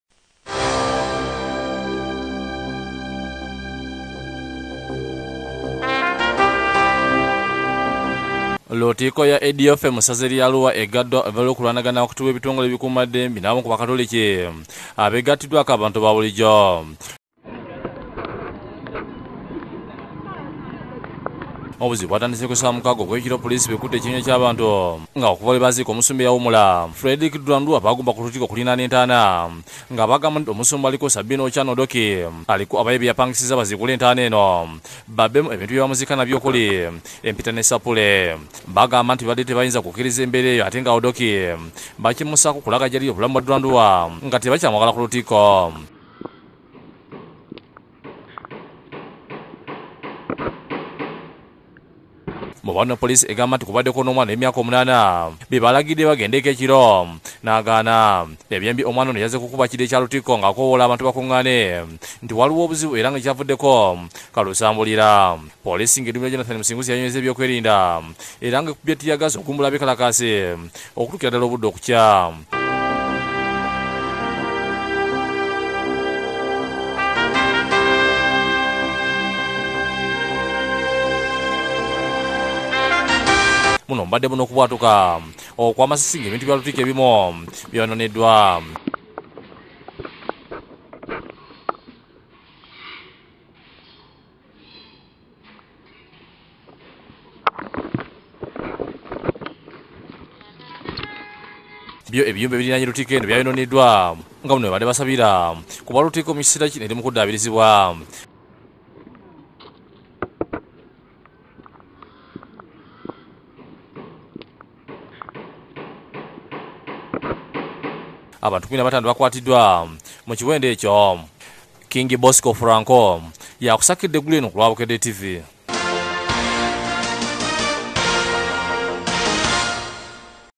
Okulwanagana ku lutikko ya Ediofe e Arua
Abakristu baagala Omusumba Odoki abaviire.